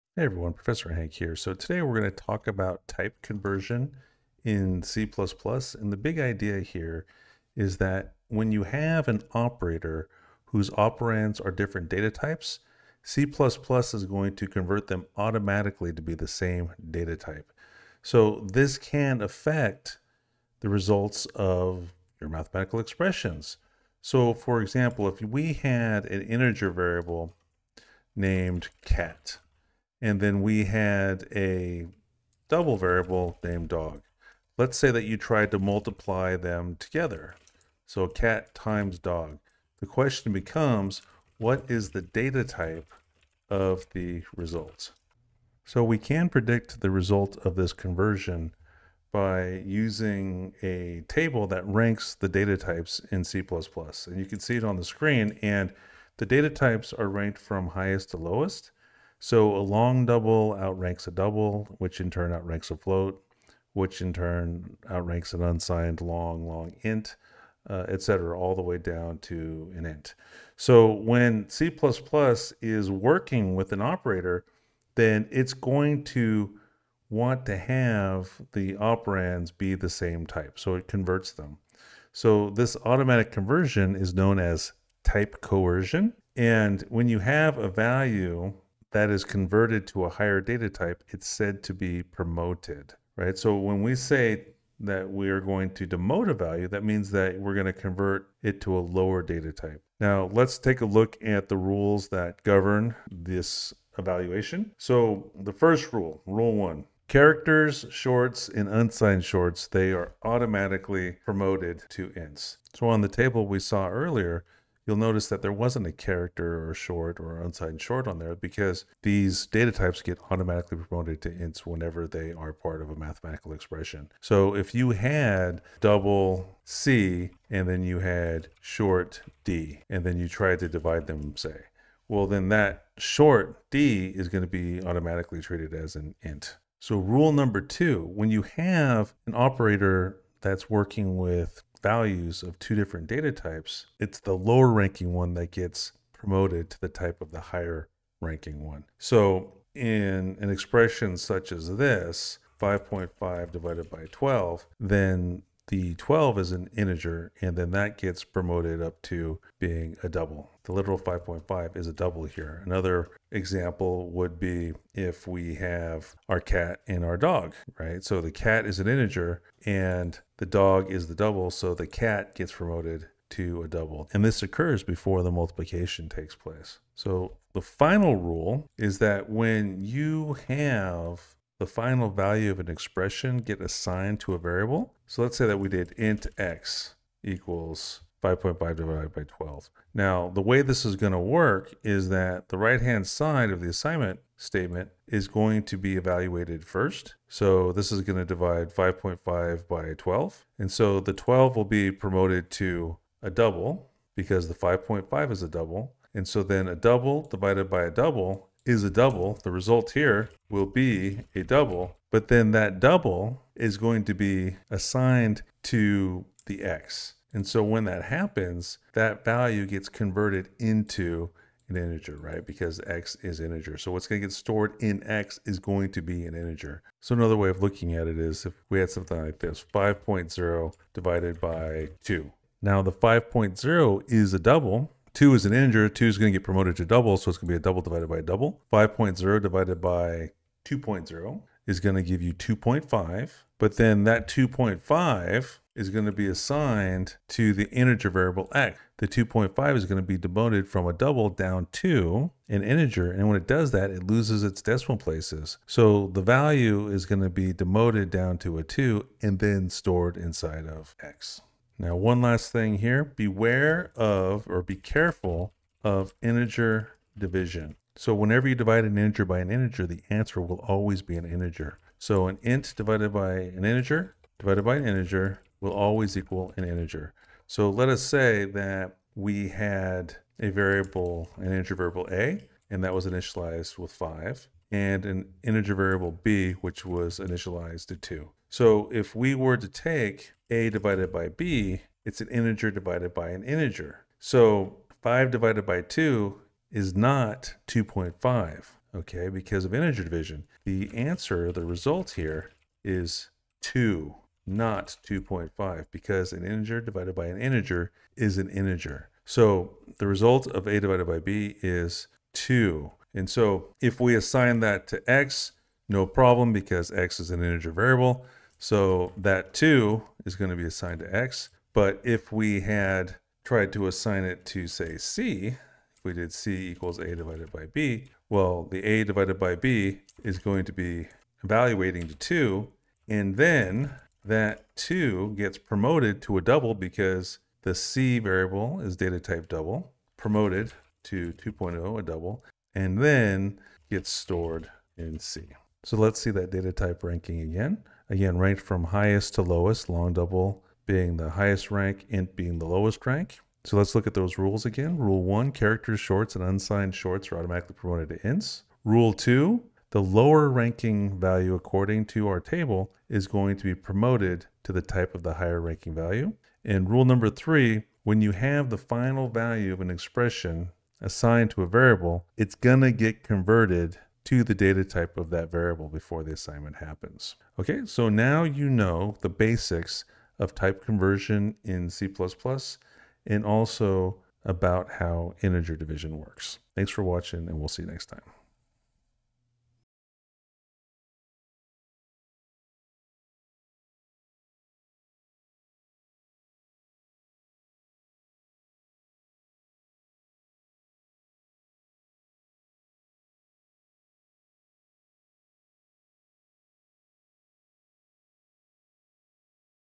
C++ type conversion and integer division [3]
C++ automatically converts operands to the same data type when they are of different types, which can have an impact on the outcomes of mathematical expressions. Learn how this works and learn about integer division in this C++ tutorial for beginners using Visual Studio 2022.

Bitcoin:  177wfkQwzXiC8o2whQMVpSyuWUs95krKYB